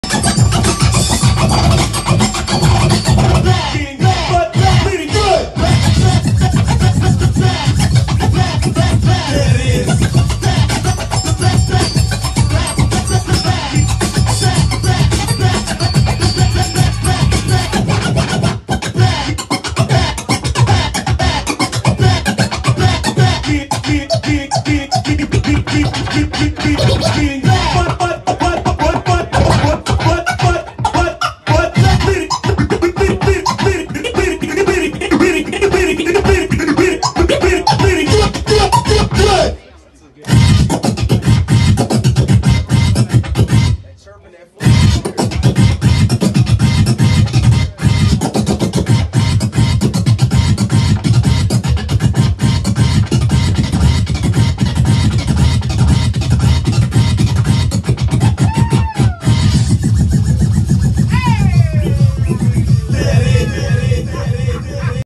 Mell Starr Is a Monster!!!!
Caught up with Mell Starr at the International DJ Cafe in Charlotte, NC. Killed my setup with the Phase needles. The man is a legend!